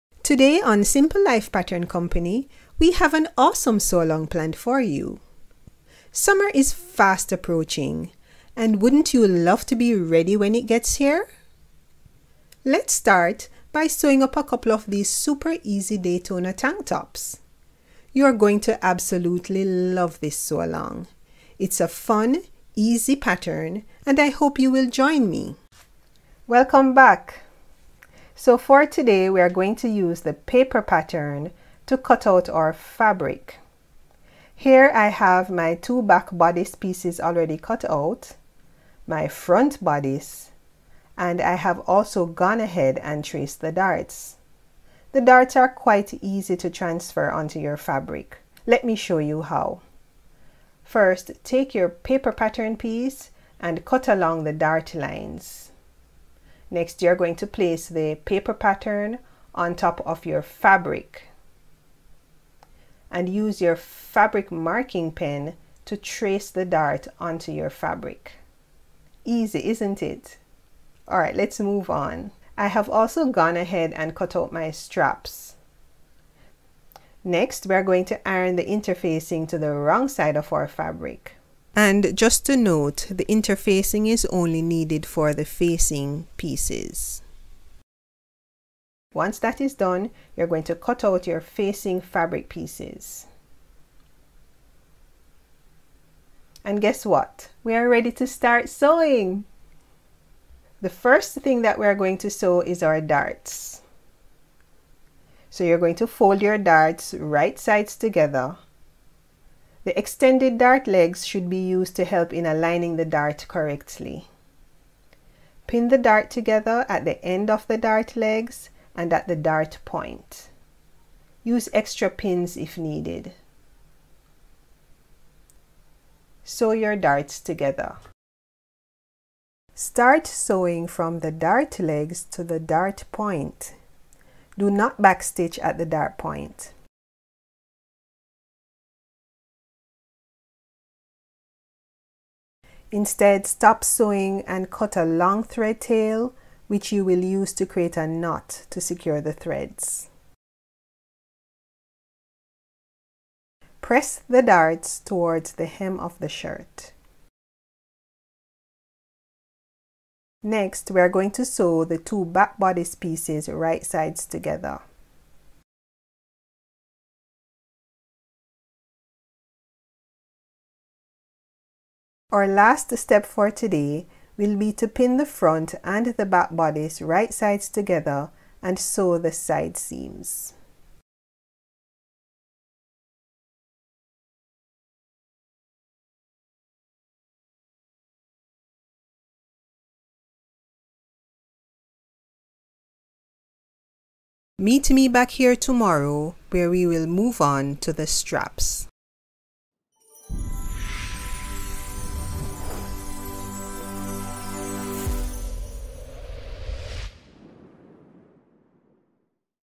Daytona Tank Top Sew Along Day 2
Day 2- Bust Darts
Come join us as we show you how to sew the comfortable Daytona women's tank top. This pattern is designed for light weight woven fabrics - so grab your favorite Rayons, voile, chiffons...This downloadable pdf pattern is great for all sewing levels. Sew along with us and join our Facebook group to show off your beautiful Simple Life Pattern Company creations, ask questions and get inspired. This video will show you step by step how to sew this tank top. Join the fun now!